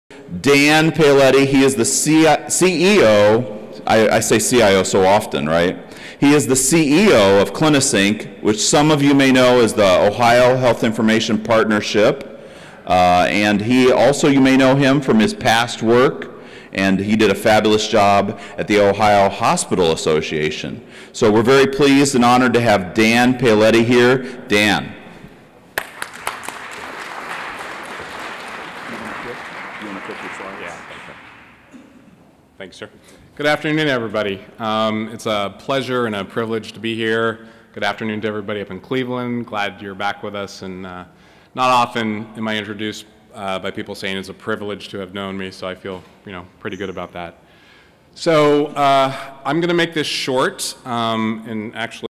David Kissinger introducing Dan Paoletti (OHIP) at OHIT Day 2014
CSOHIMSS President Elect David Kissinger introducing Dan Paoletti, CEO of the Ohio Health Information Partnership, at OHIT Day 2014.